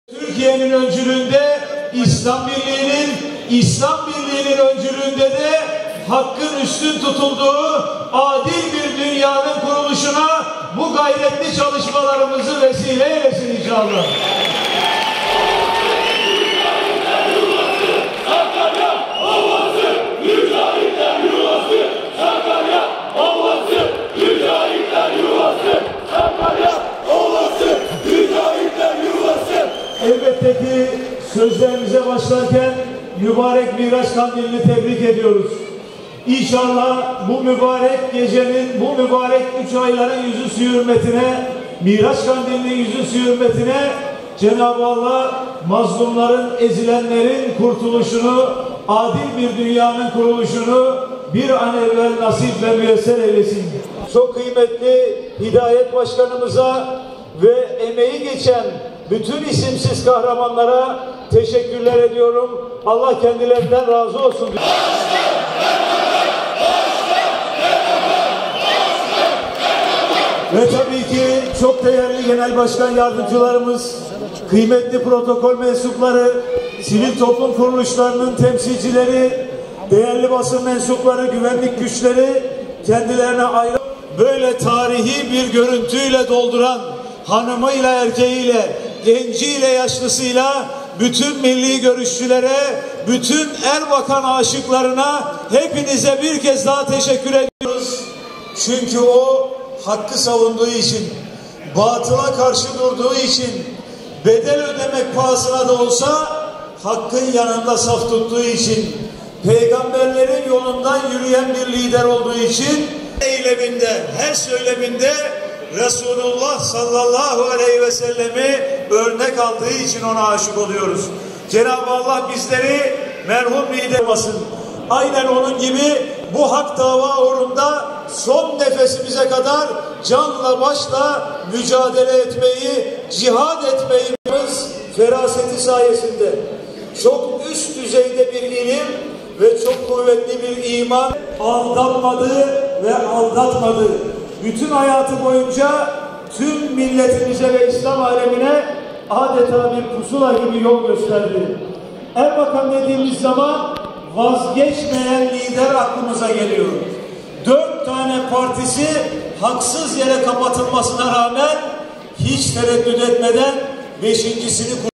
Necmettin Erbakan ölümünün 11'nci yıl dönümünde anılıyor
Erbakan Vakfı Sakarya Şubesi'nin düzenlediği "Nesillere İstikamet Veren Erbakan" programı Atatürk Kapalı Spor Salonu'nda gerçekleşiyor.